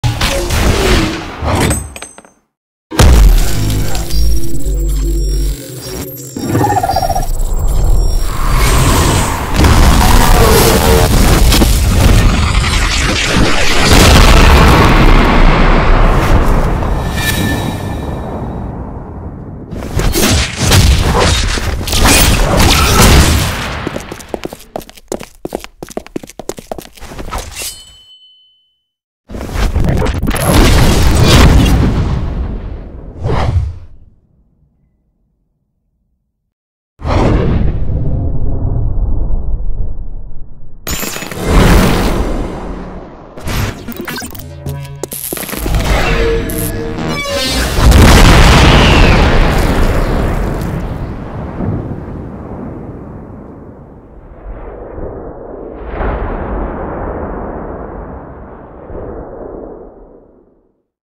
Danger Zone 2 Joint | Walt's Part | Stick Nodes
hey guys, finally the sfx has been finished for the whole joint, this is only my part, my jointmates are

Thank you Peirce for sounding the joint 🙏

probably the best animation i have ever done, theres gonna something else thats gonna top it tho soon.